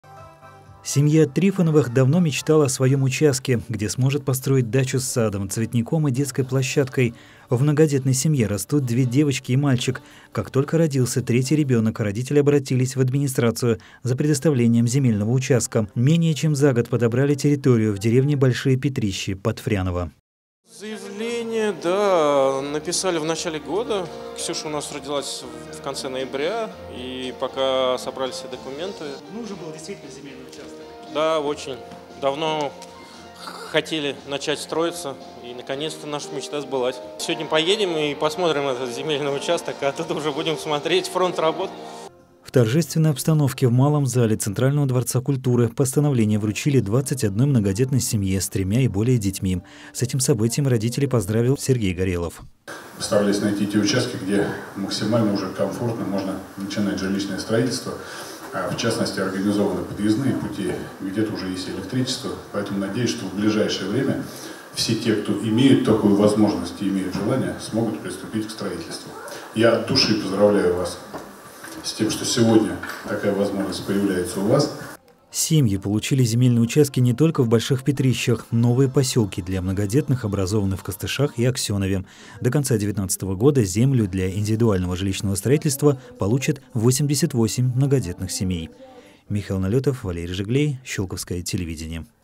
21 многодетная семья из Щёлково получила земельные участки
На торжественное вручение молодые родители приехали в полном составе. Подробности расскажет наш корреспондент.